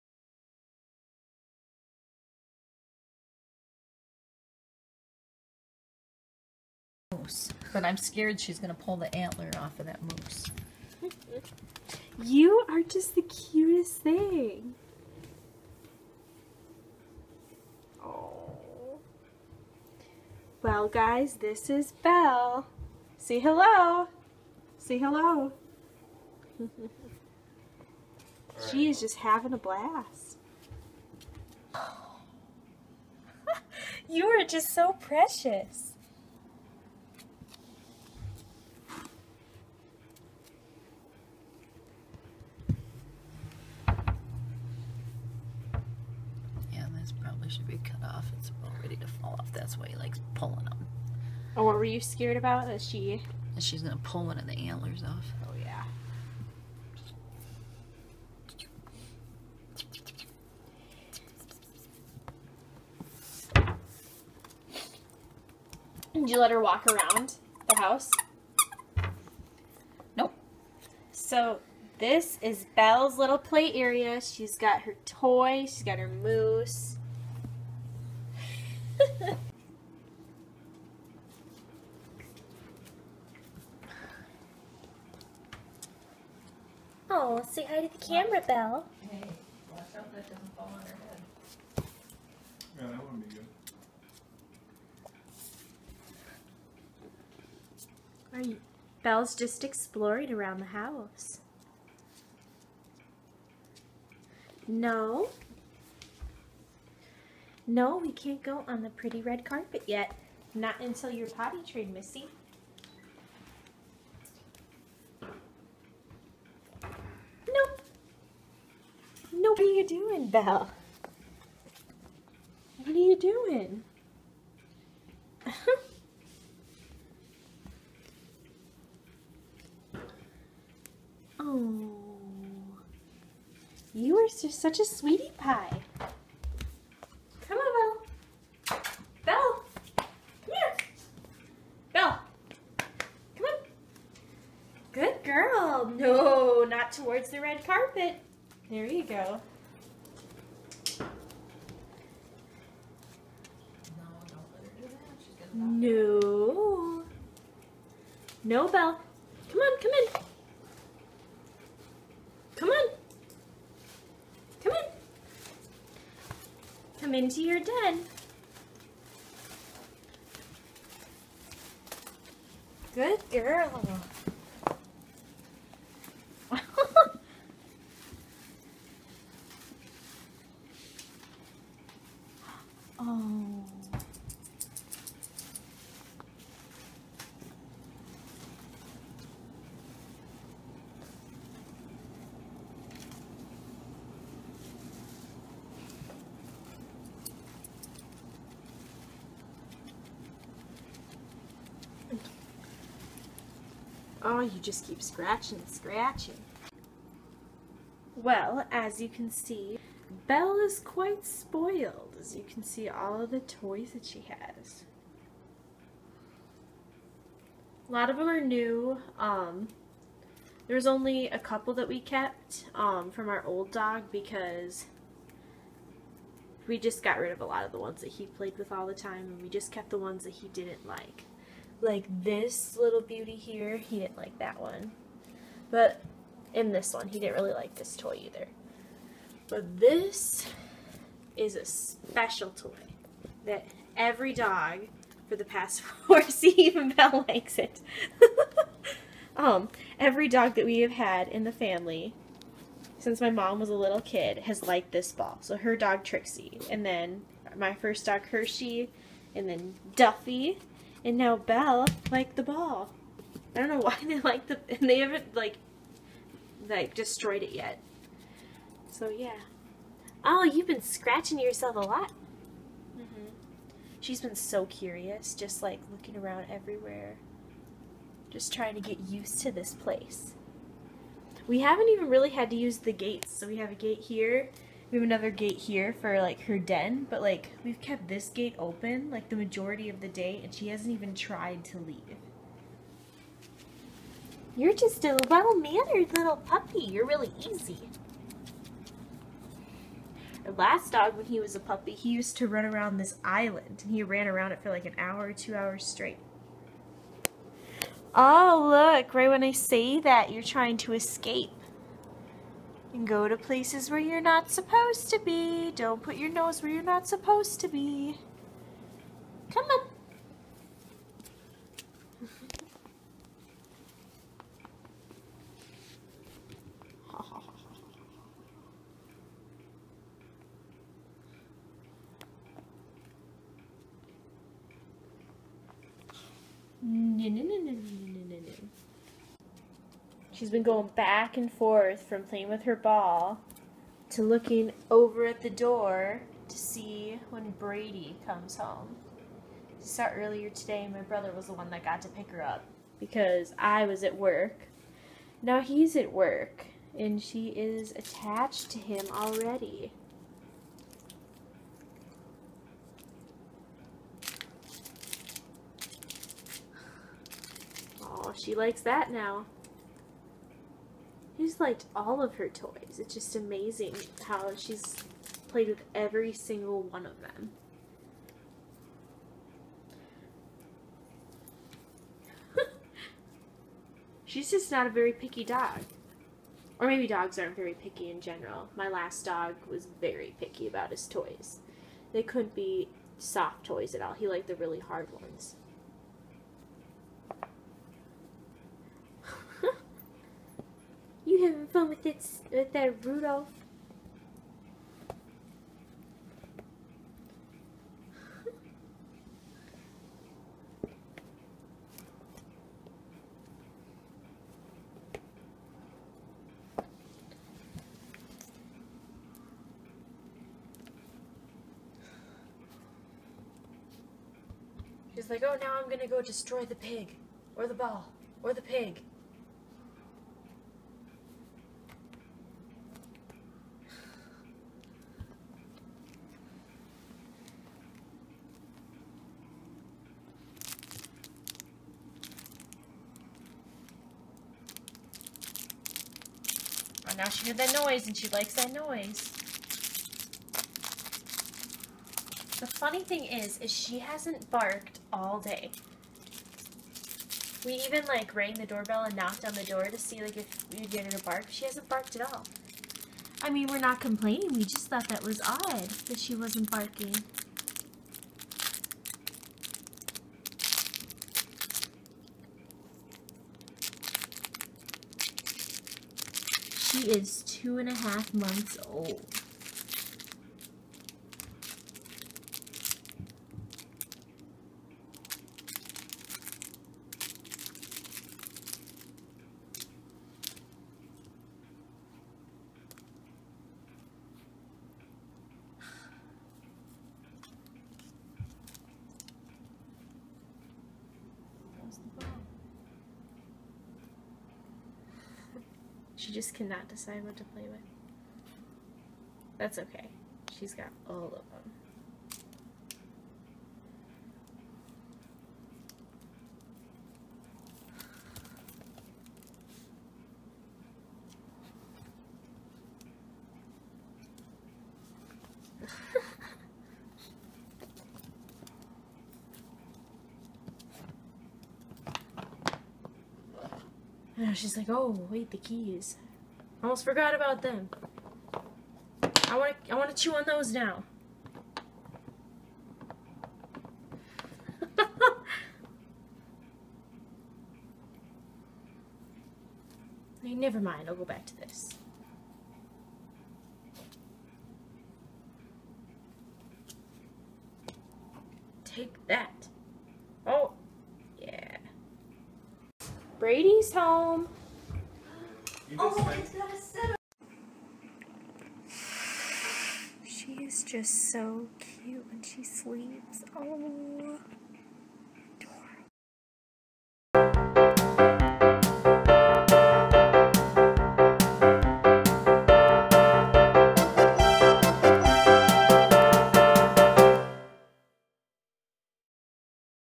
THE NEW PUPPY!!!!!!!!!!!!!
This is the video about my families new dog Belle.  She is a mini wirehaired dachshund.